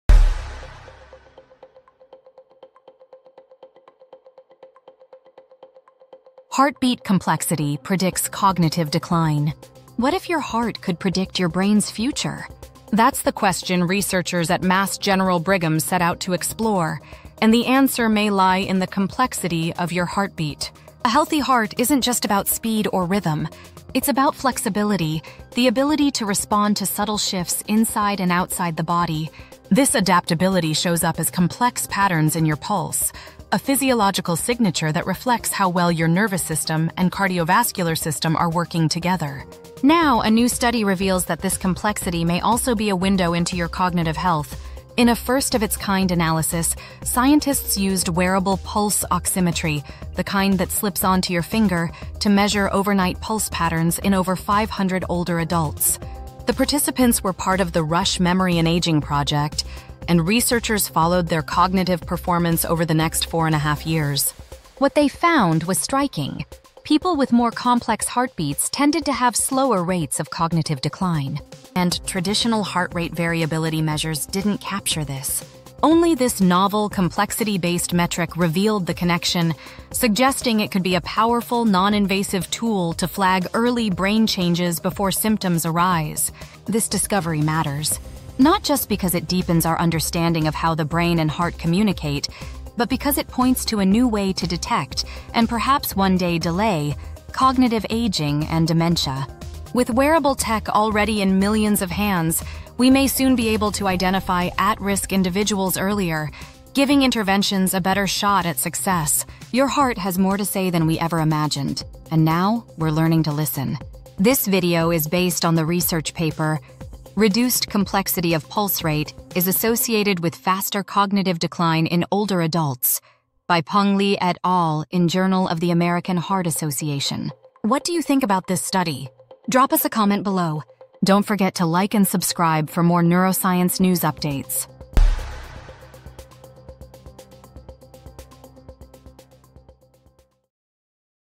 Heartbeat Complexity Predicts Cognitive Decline
A new study from Mass General Brigham researchers reveals that the complexity of pulse rate — measured via overnight wearable oximetry — can predict future cognitive decline in older adults. This novel heart rhythm metric, more sensitive than traditional heart rate variability, may serve as a noninvasive tool for early detection of brain aging and dementia risk. The findings underscore the intimate link between cardiovascular adaptability and cognitive resilience, offering new insights into the physiology of healthy aging.